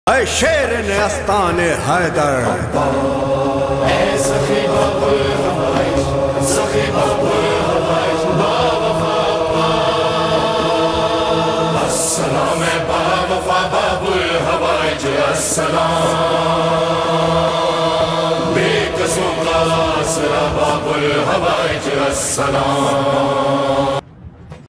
Whatsapp Status Mola Abbas Mola Ghazi Abbas Whatsapp Status || Ghazi Abbas Alam || Muharram
Beautiful black jalali alam e Ghazi Abbas Alamdaar a.s in Karachi Chehlum Juloos 2021.